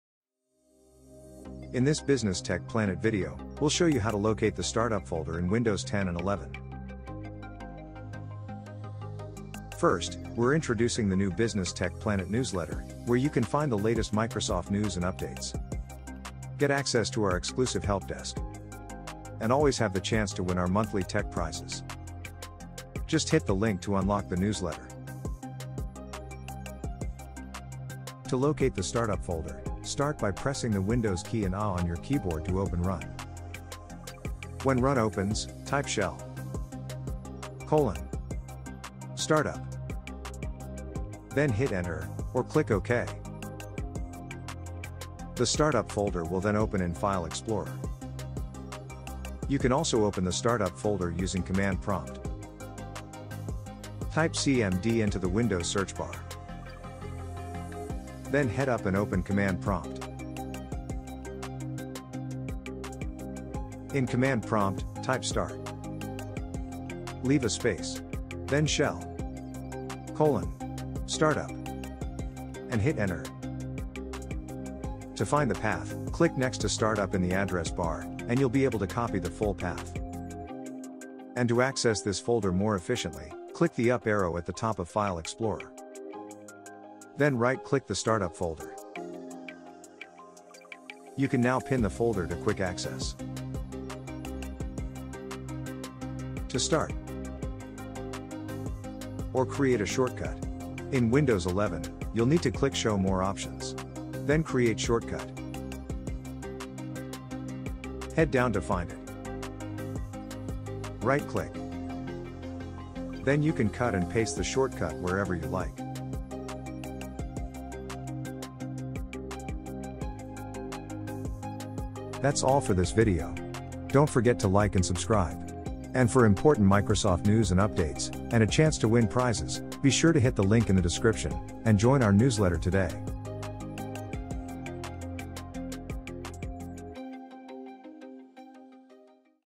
Location of the STARTUP folder in Windows 11/10
Where is the startup folder in Windows 11 / 10?!

In this video, we'll show you how to locate the startup (start-up) folder in Windows 10 and 11. We'll do this using Run and Command Prompt.

In Run:

shell:startup

In Command Prompt:

start shell:startup